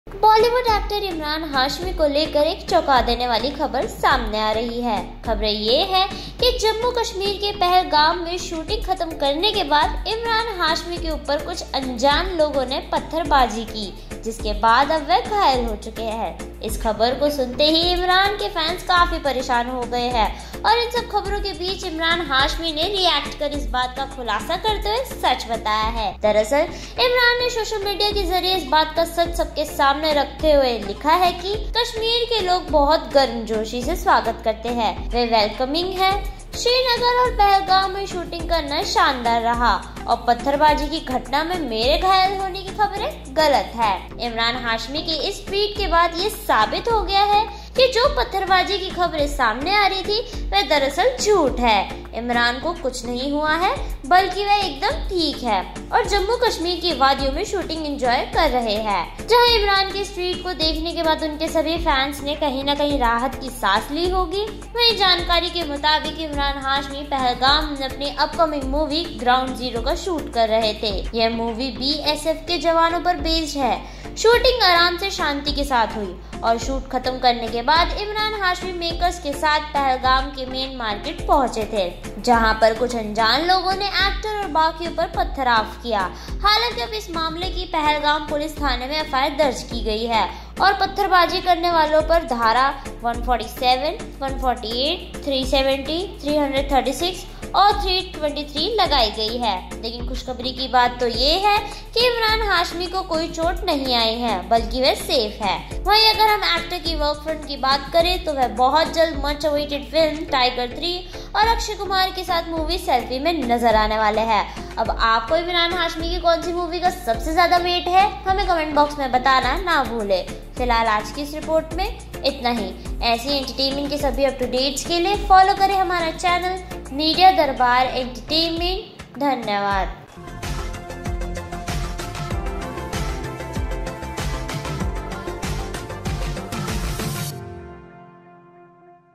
Stone pelting on Emraan Hashmi in Jammu-Kashmir, information given to fans in critical condition
Stone pelting on Emraan Hashmi in Jammu-Kashmir, information given to fans in critical condition | Media Darbar Entertainment

बॉलीवुड एक्टर इमरान हाशमी को लेकर एक चौका देने वाली खबर सामने आ रही हैं खबरे ये है कि, जम्मू-कश्मीर के पहलगाम में शूटिंग खत्म करने के बाद इमरान हाश्मी के ऊपर कुछ अनजान लोगों ने पत्थरबाजी की जिसके बाद अब वह घायल हो चुके हैं। इस खबर को सुनते ही इमरान के फैन्स काफी परेशान हो गए हैं। और इन सब खबरों के बीच इमरान हाश्मी ने रियेक्ट कर इस बात का खुलासा करते हुए सच बताया हैं।

Written By- Palak Mujral
Edit By- Punit Verma